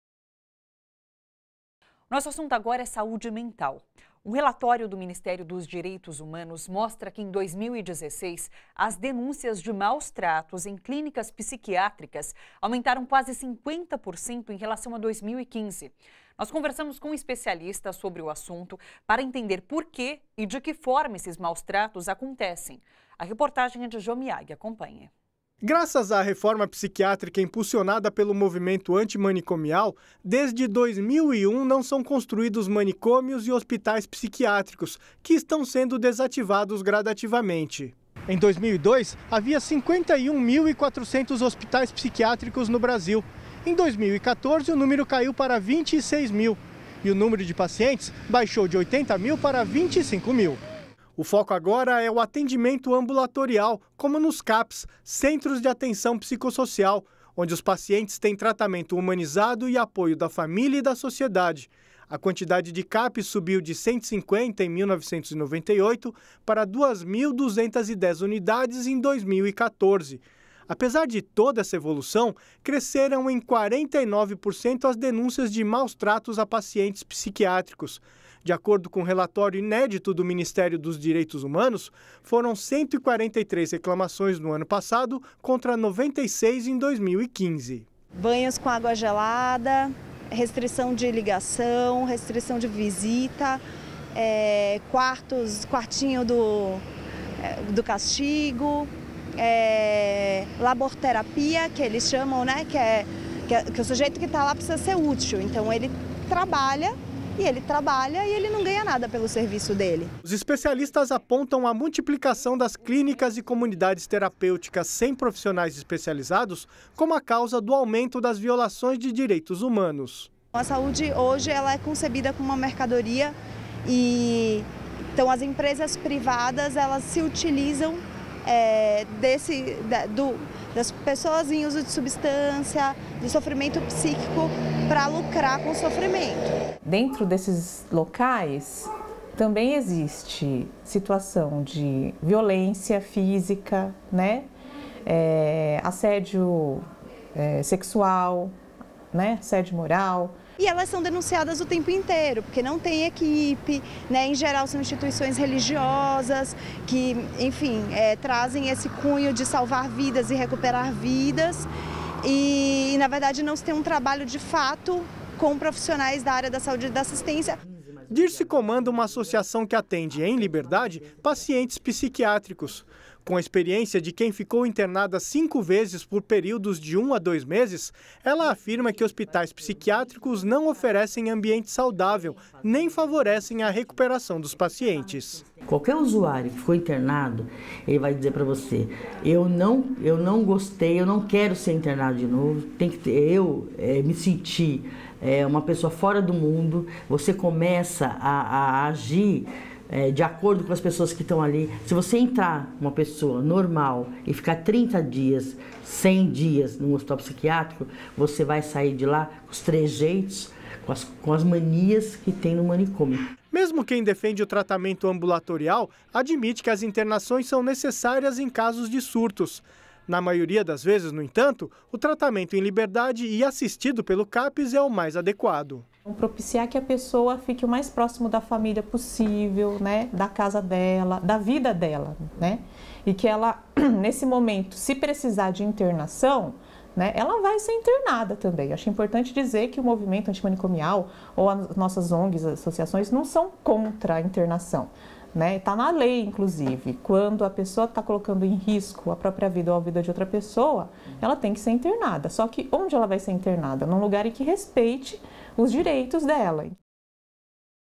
Denúncias de maus-tratos em clínicas psiquiátricas crescem em 50% em um ano
Um relatório do Ministério dos Direitos Humanos mostra que em 2016, as denúncias de maus-tratos em clínicas psiquiátricas aumentaram quase 50% em relação a 2015. Conversamos com especialistas sobre o assunto para entender por que e de que forma esses maus-tratos acontecem.